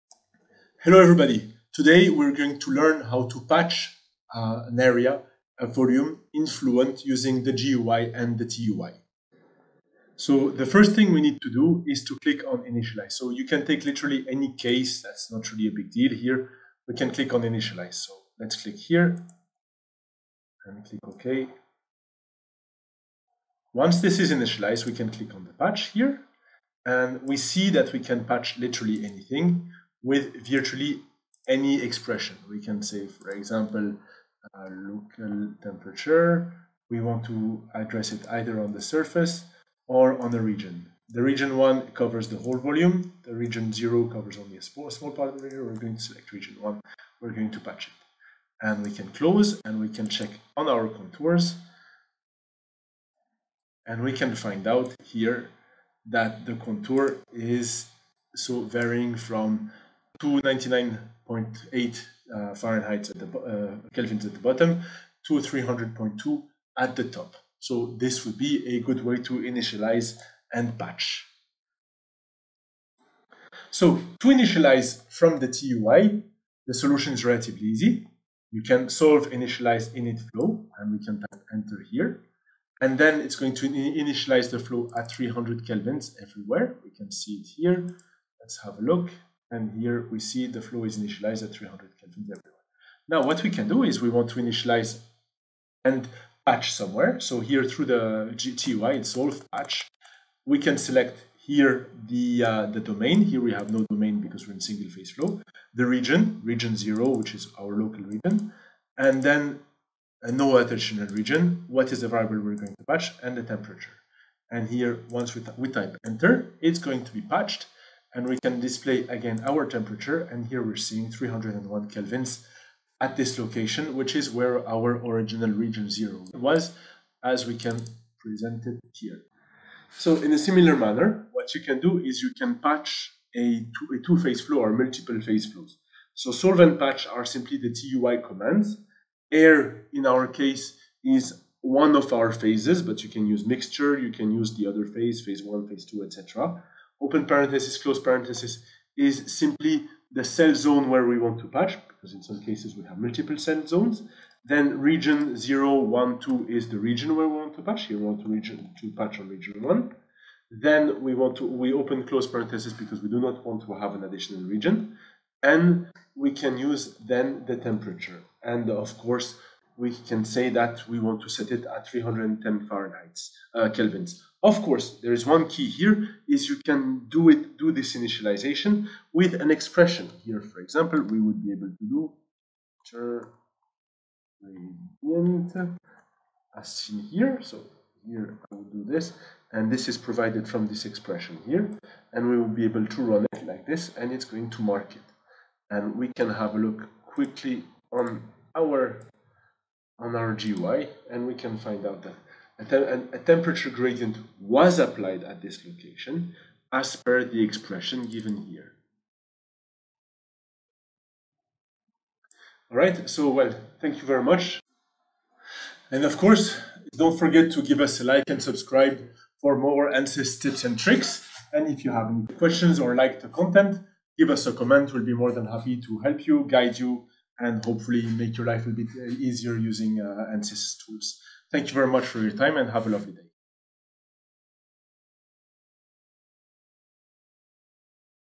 How to Patch in Ansys Fluent Through the GUI and the TUI
Learn how to patch in Ansys Fluent through the GUI and the TUI